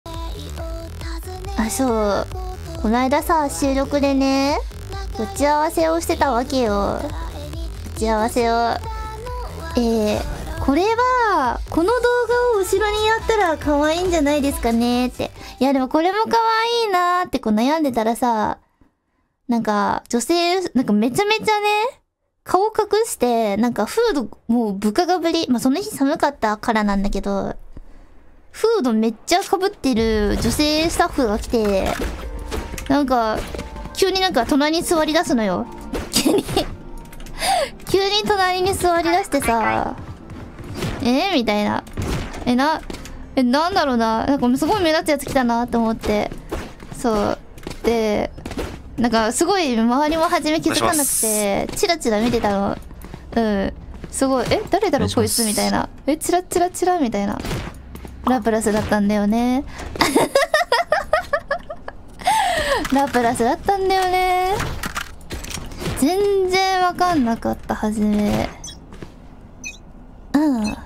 Random Girl Sits Next To Roboco IRL and Ends Up Being A HoloMember...【Hololive】
使用BGM
□DOVA-SYNDROME